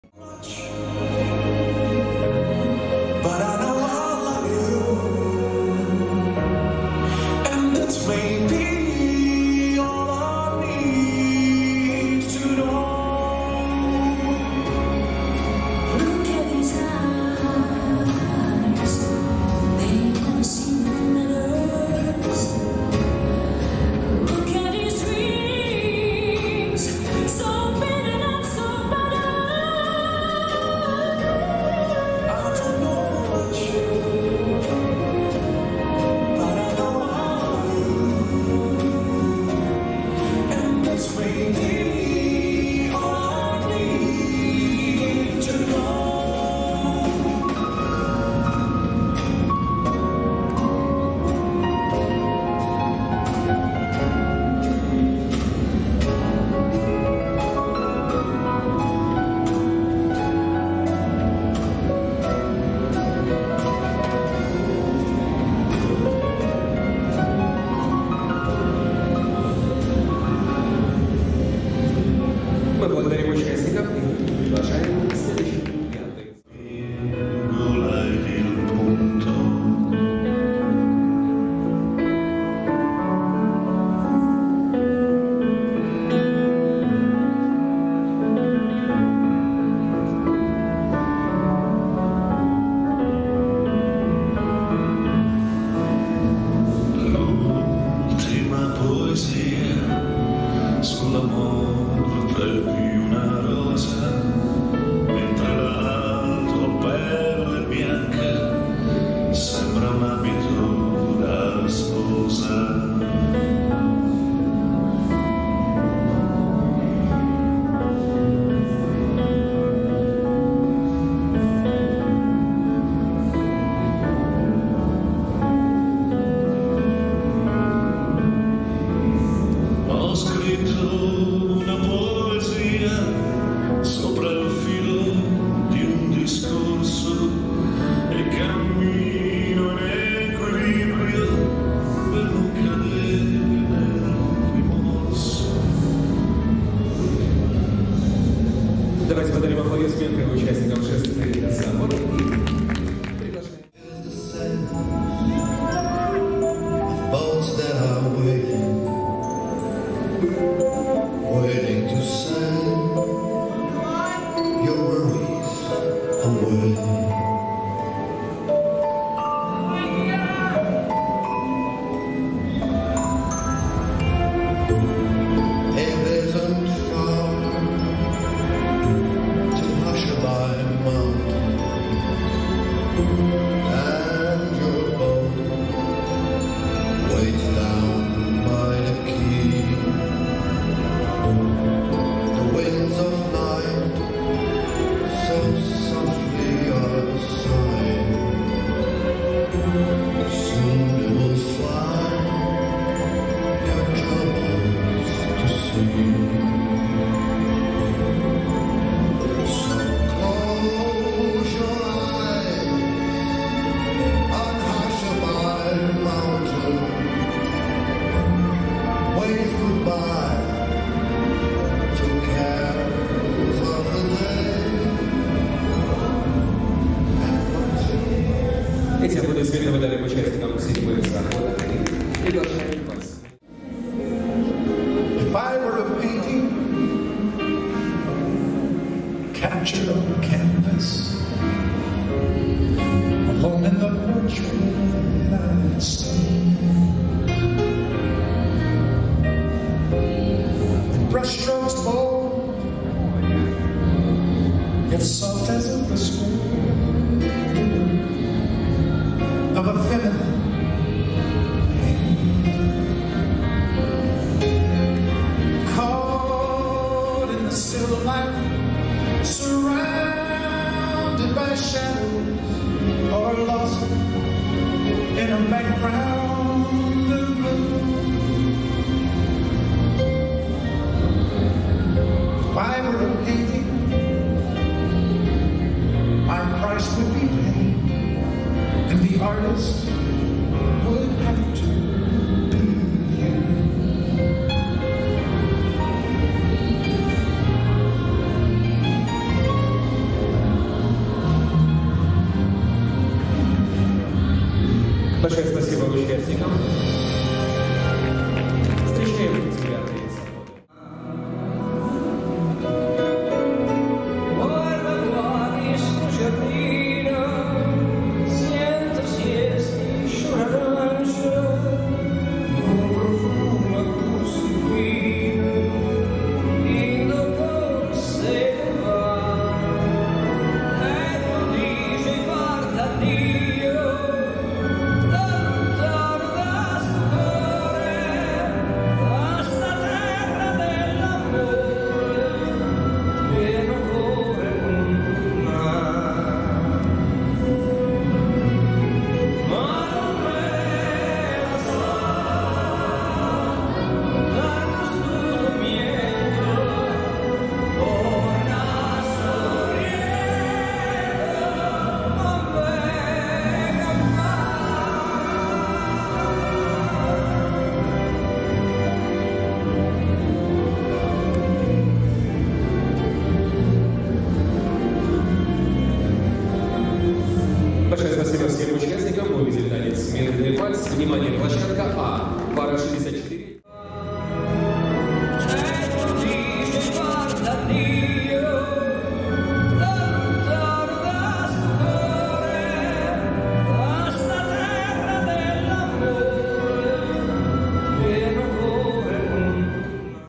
Waltz Compilation = 2022 Cup of Russia Adult Ballroom 2Round
Slow Waltz 2Round Compilation
2022 Cup of Russia = Amateur (Adult) Ballroom

Beautiful couples perform the "Waltz" dance at the 2022 Cup of Russia Amateur (Adult) Ballroom tournament:
0:00 - Pavel Zakharenko & Alisa Busurova /57
1:16 - Alexey Kurakin & Lidia Titova /90
2:34 - Danila Boriskin & Polina Kulakova /30
3:53 - Daniil Proklov & Anna Kostrygina /125
5:10 - Valentin Desinov & Anna Vaniashkina /47

June 20, 2022, Moscow

Ranking Final:
1. Alexey Glukhov & Anastasia Glazunova
2. Evgeny Nikitin & Anastasia Miliutina
3. Ivan Varfolomeev & Yana Masharova
4. Ivan Reshetnikov & Elizaveta Kharinova
5. Evgeny Sveridonov & Angelina Barkova
6. Kirill Vlasov & Kristina Polunina